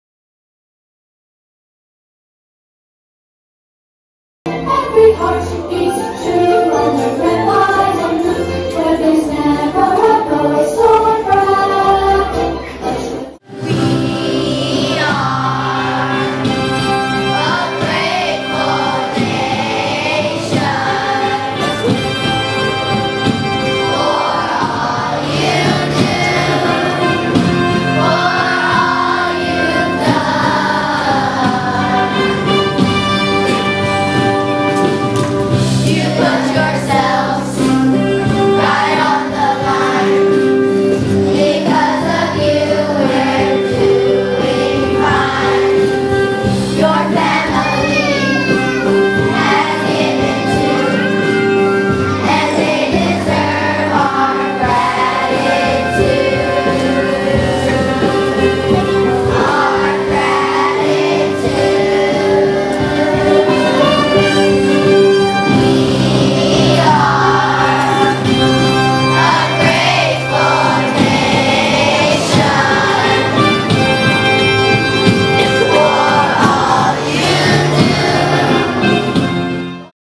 2019 Veterans Day at Bastrop ISD
Our campuses celebrated and honored local Veterans through music and historical performances. Watch the video to see photos and music from today's events. Thank you, Veterans!